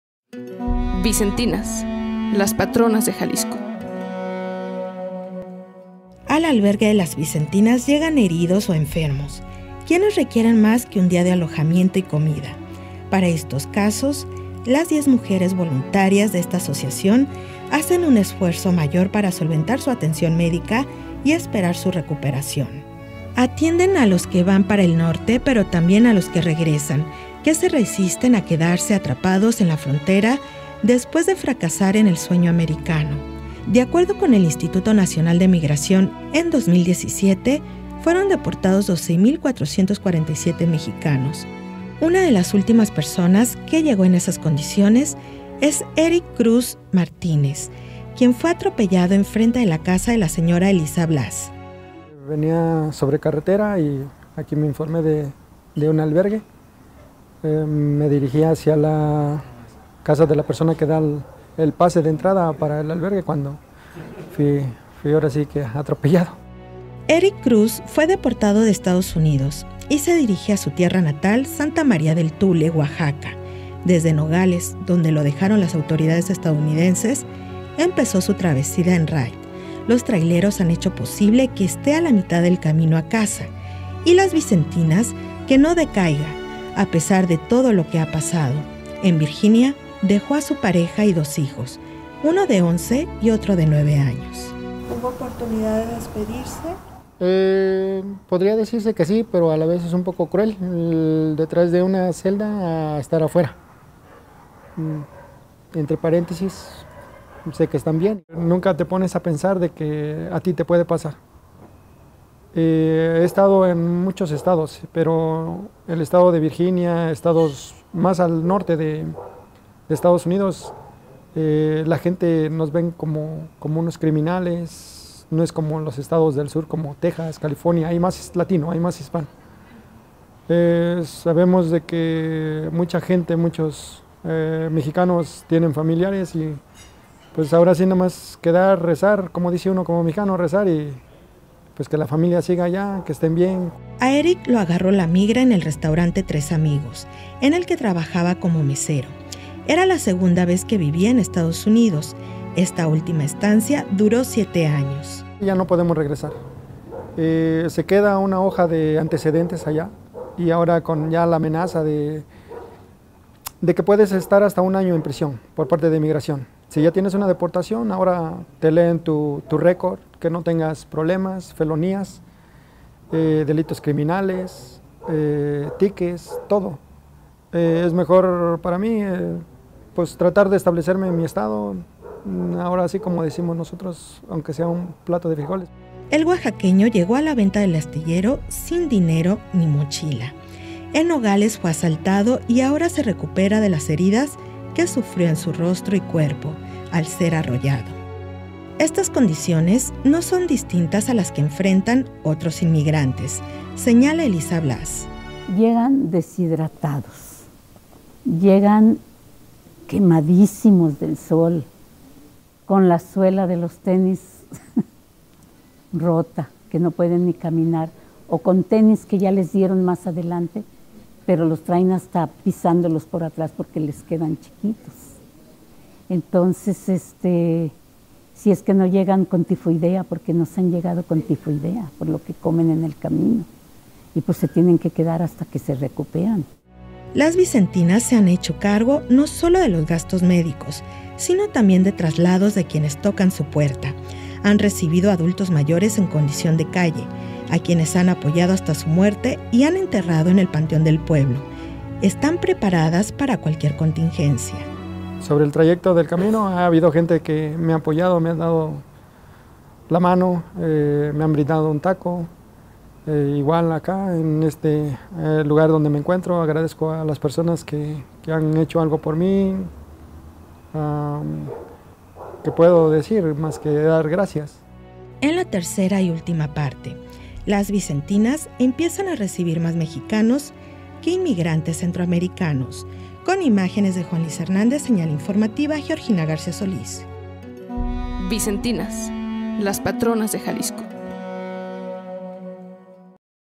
Especial | Las Vicentinas - Parte 2
Las Vicentinas 2:  Los migrantes llegan al albergue de Las Vicentinas enfermos o heridos; en estos casos la ayuda se extiende hasta que se recuperen.